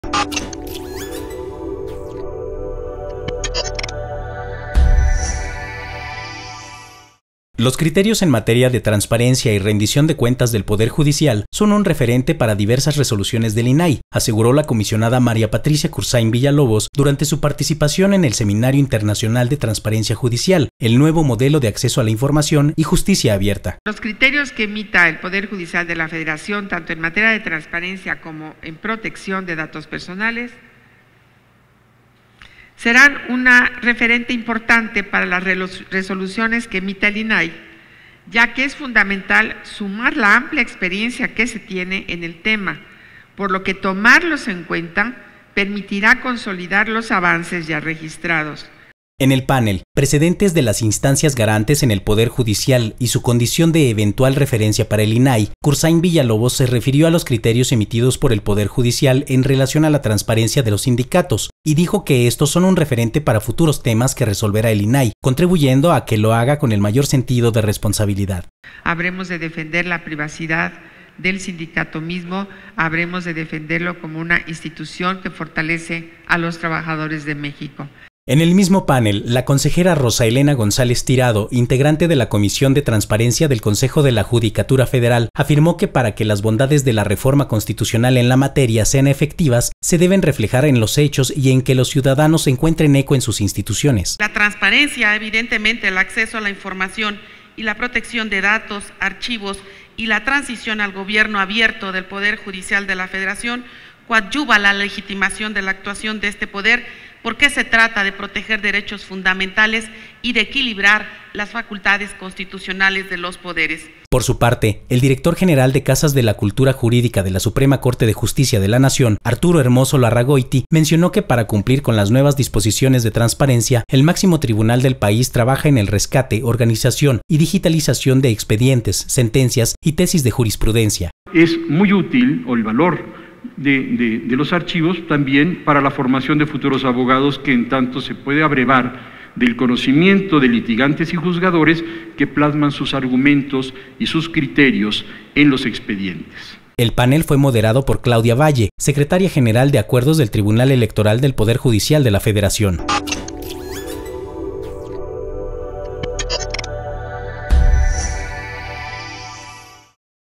INAI Criterios del Poder Judicial en transparencia son referente para resoluciones del INAI MPKV
 La comisionada del INAI participó en el
Seminario Internacional de
Transparencia Judicial
 Planteó impulsar el diálogo y la
cooperación entre el Poder Judicial y el
Instituto, ya que los criterios
desarrollados por los órganos
jurisdiccionales constituyen un
verdadero proceso dialéctico para el
INAI